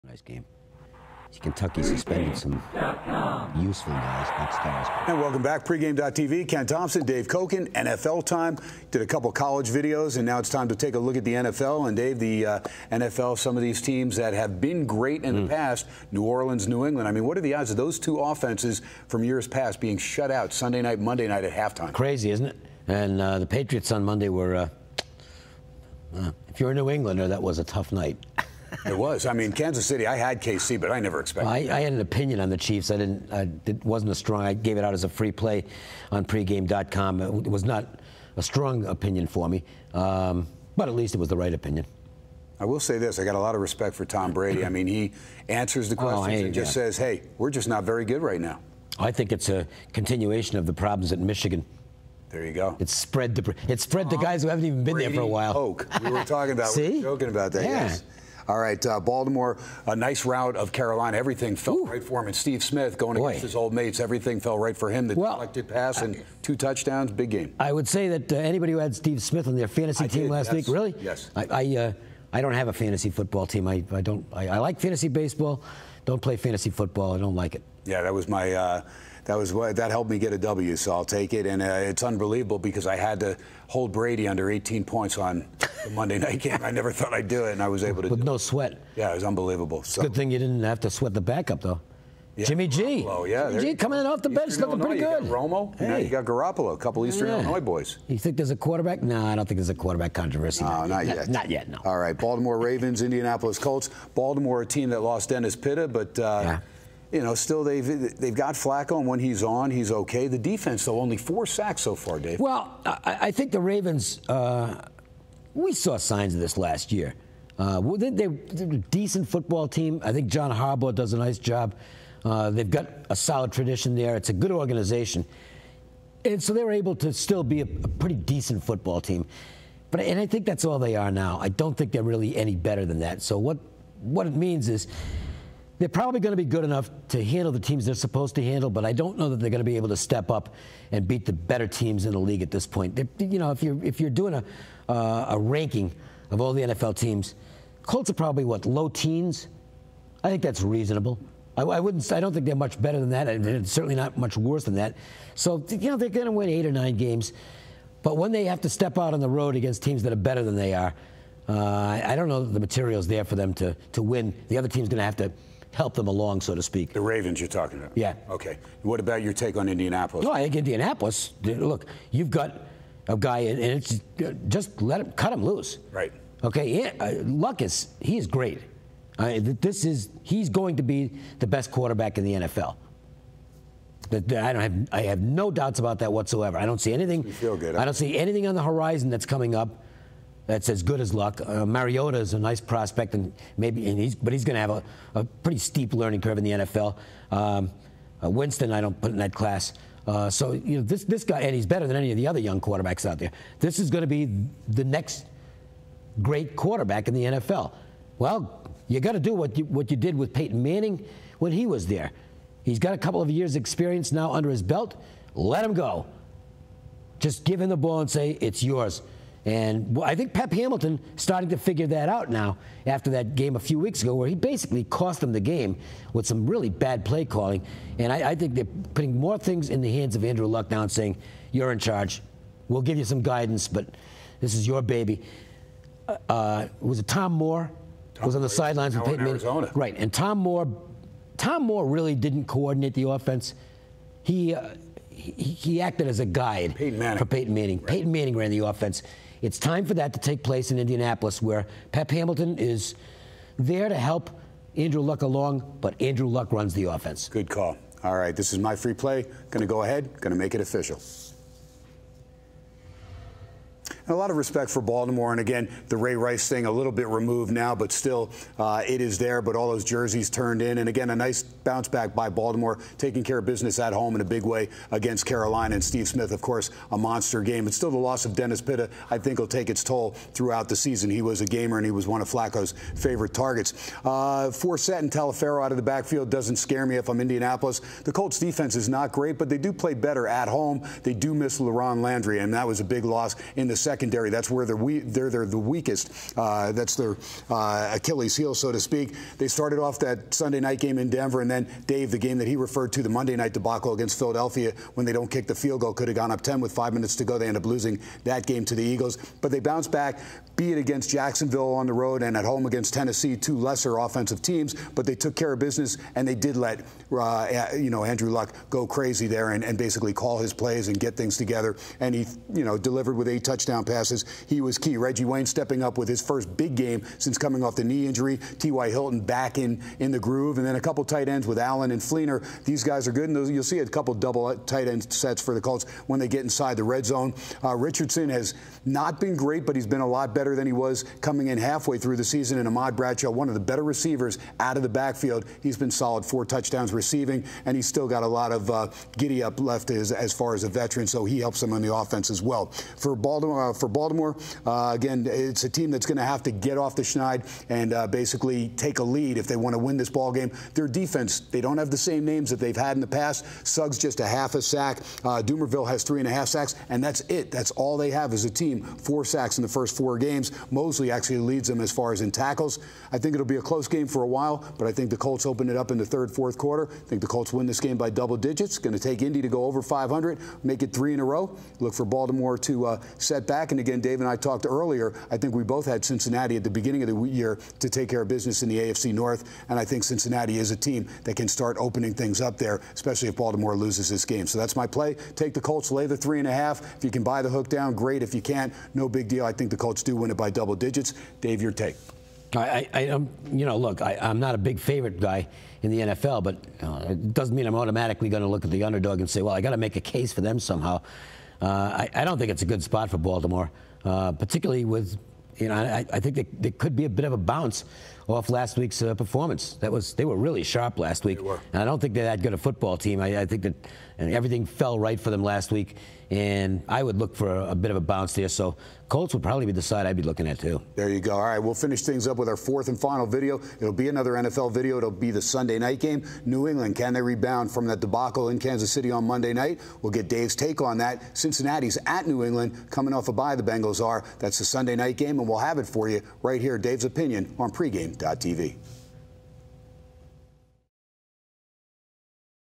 NFL Free Picks: Baltimore Ravens vs Indianapolis Colts Betting
What pro football bettors need to know about betting Baltimore Ravens vs Indianapolis Colts on Sunday (10/05/14) - a free pick from Pregame.com's Ken Thomson with added Insight from Dave Cokin.